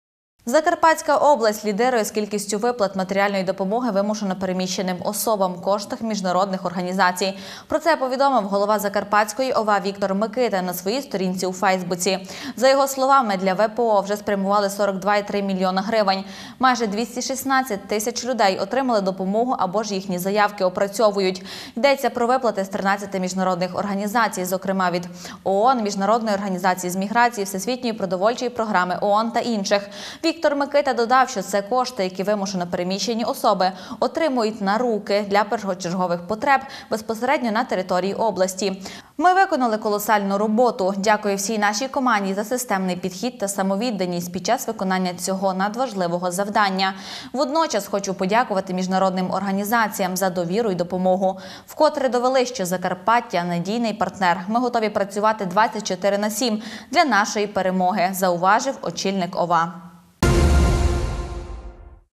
Лідер з виплат міжнародної допомоги ВПО
Закарпатська область лідирує за кількістю виплат матеріальної допомоги вимушено переміщеним особам коштом міжнародних організацій. Про це повідомив голова ОВА Віктор Микита на своїй сторінці у фейсбуці. За його словами, для ВПО вже спрямували 42,3 млн грн. Майже 216 тисяч людей отримали допомогу або ж їхні заявки опрацьовують. Йдеться про виплати від 13 міжнародних організацій, зокрема від УВКБ ООН, Міжнародної організації з міграції, Всесвітньої продовольчої програми ООН та інших. Віктор Микита додав, що це кошти, які вимушено переміщені особи отримують «на руки» для першочергових потреб безпосередньо на території області. «Ми виконали колосальну роботу. Дякую всій нашій команді за системний підхід та самовідданість під час виконання цього надважливого завдання. Водночас хочу подякувати міжнародним організаціям за довіру й допомогу. Вкотре довели, що Закарпаття — надійний партнер, ми готові працювати 24/7 для нашої перемоги», — зауважив очільник ОВА.